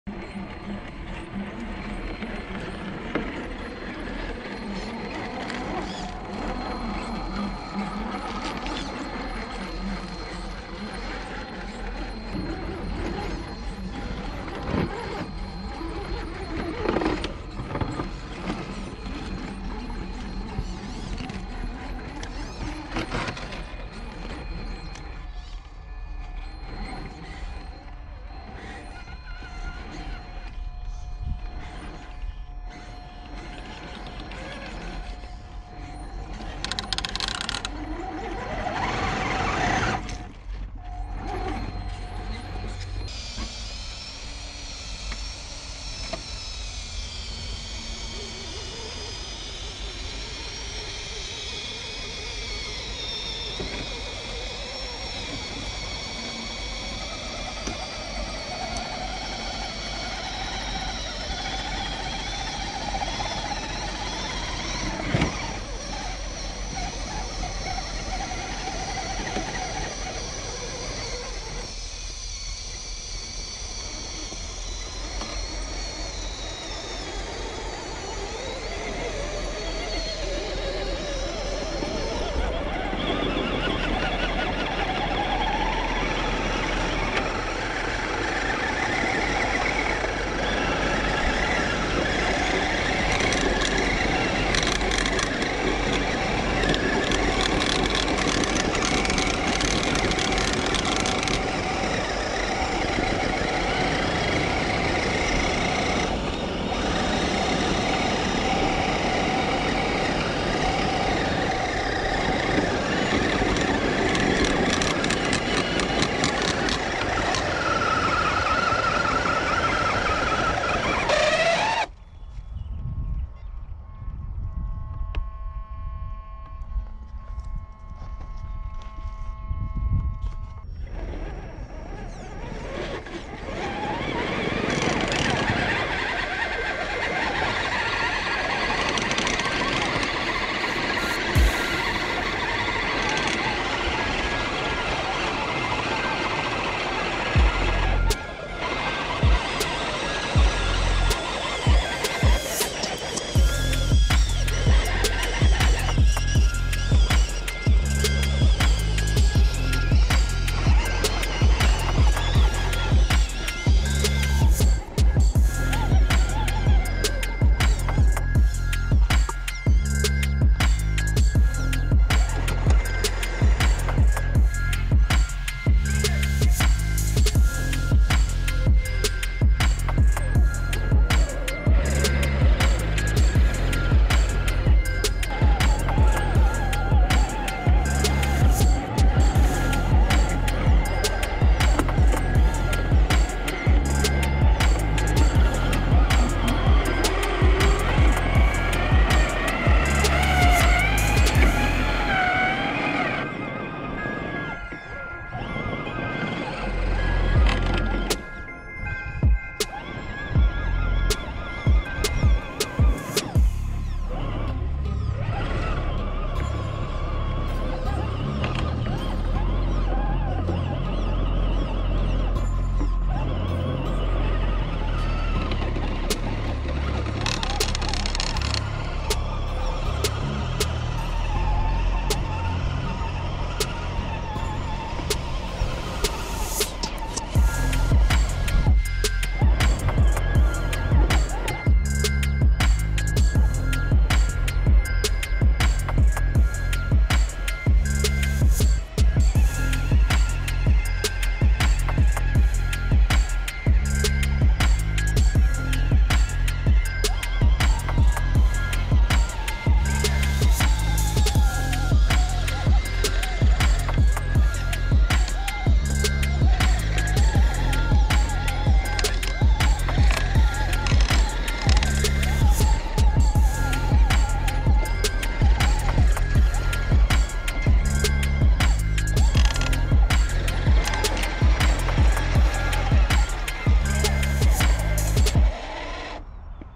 Два внедорожника вытаскивают FJ Cruiser из каньона! Жесткий OFFroad
FJ Cruiser застрял в коньоне. Ford F-100 и ЗИЛ 650 сил вытаскивают его.